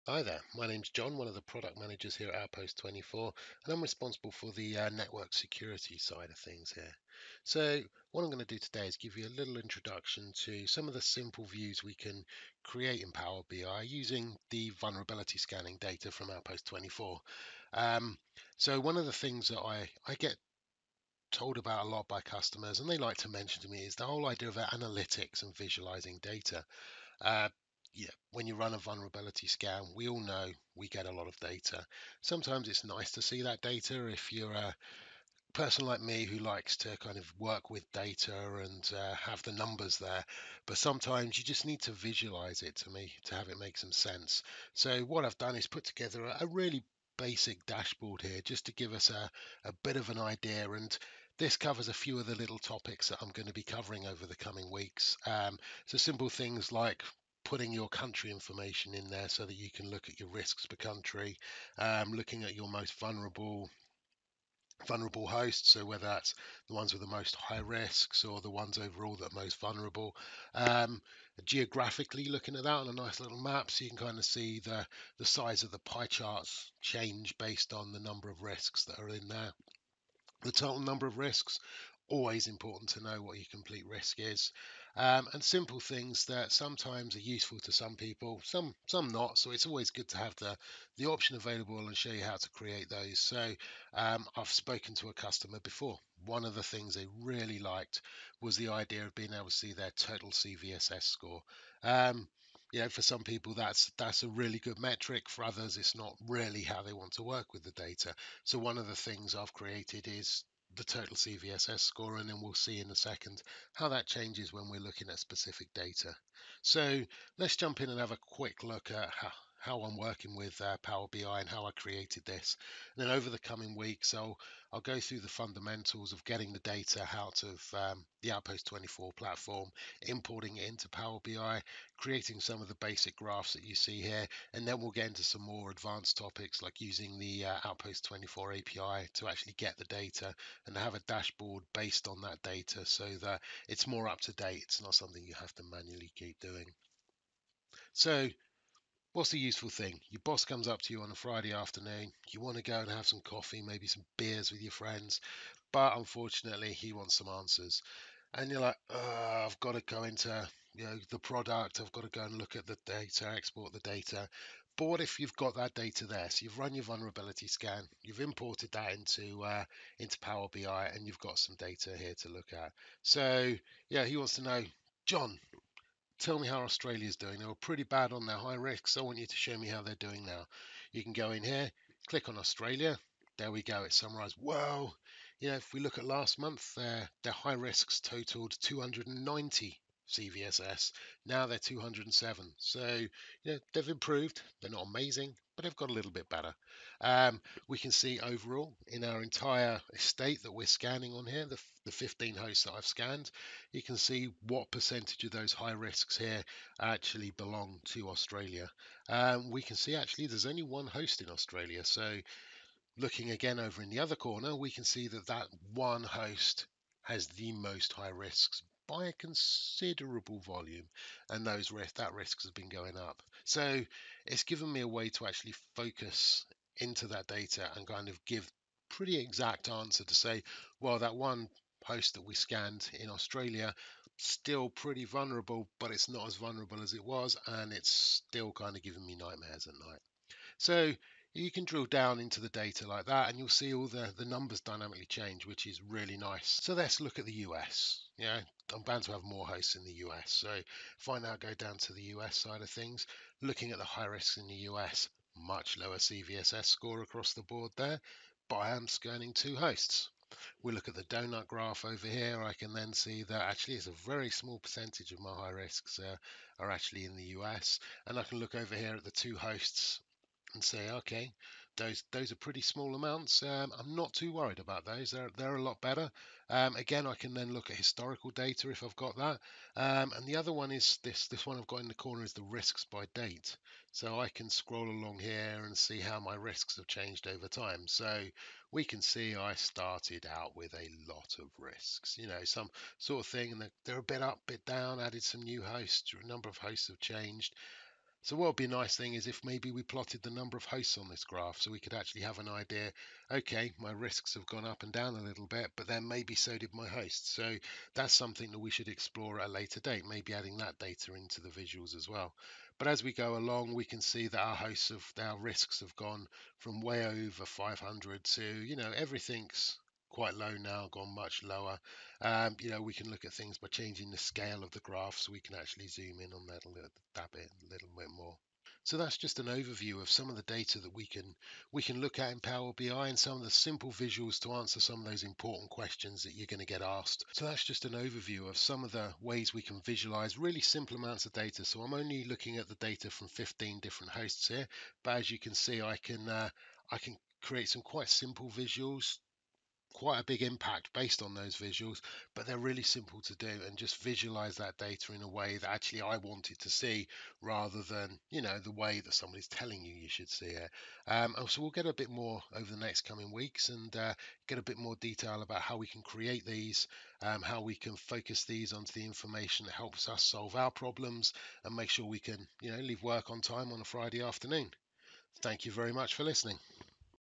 Outpost24 and Microsoft Power BI
Discover how to leverage Outpost24 and Microsoft Power BI to add clarity to your vulnerability data and deliver insights to enable fast, informed decisions about your cyber exposure.